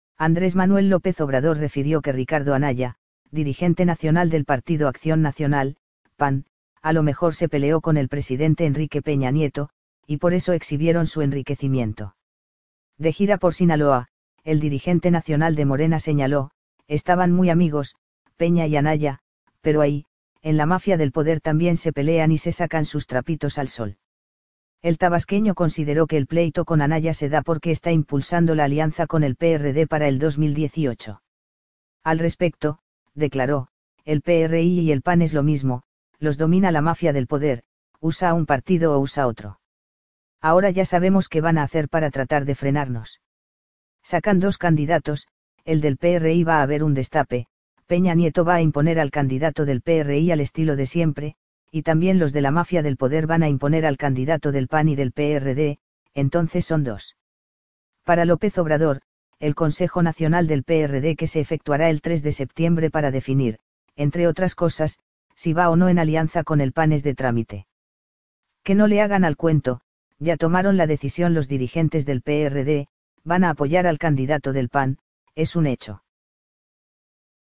Anaya se peleó con Peña y por eso le sacaron sus “trapitos al sol”!
- Andrés Manuel López Obrador refirió que Ricardo Anaya, dirigente nacional del Partido Acción Nacional (PAN), a lo mejor se peleó con el presidente Enrique Peña Nieto, y por eso exhibieron su enriquecimiento.

De gira por Sinaloa, el dirigente nacional de Morena señaló: “estaban muy amigos (Peña y Anaya), pero ahí, en la mafia del poder también se pelean y se sacan sus trapitos al sol”.

El tabasqueño consideró que el pleito con Anaya se da porque está impulsando la alianza con el PRD para el 2018.

Al respecto, declaró: “El PRI y el PAN es lo mismo, los domina la mafia del poder, usa a un partido o usa otro. Ahora ya sabemos qué van a hacer para tratar de frenarnos. Sacan dos candidatos: el del PRI va a haber un destape, Peña Nieto va a imponer al candidato del PRI al estilo de siempre, y también los de la mafia del poder van a imponer al candidato del PAN y del PRD, entonces son dos”.

Para López Obrador, el Consejo Nacional del PRD que se efectuará el 3 de septiembre para definir, entre otras cosas, si va o no en alianza con el PAN es de trámite.

“Que no le hagan al cuento, ya tomaron la decisión los dirigentes del PRD, van a apoyar al candidato del PAN, es un hecho”.